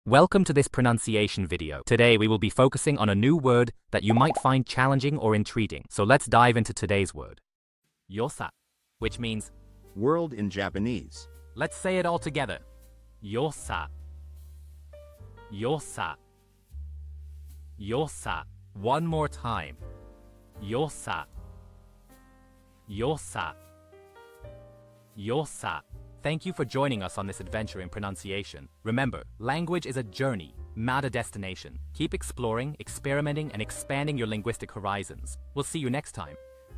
How to pronounce yosa | 世さ (World in Japanese)
Master the Pronunciation of 'yosa | 世さ' - which means : World in Japanese 📝 with @PronunciationJapanese 🎓🗣️ - [Your Guide to Japanese]

Learn to pronounce 'yosa' perfectly in Japanese with our easy and engaging tutorial by @PronunciationJapanese! 📖📽️ In this comprehensive video, we provide you with native speaker examples 🗣️👂 and phonetic breakdowns 🎵 to ensure you get the pronunciation just right.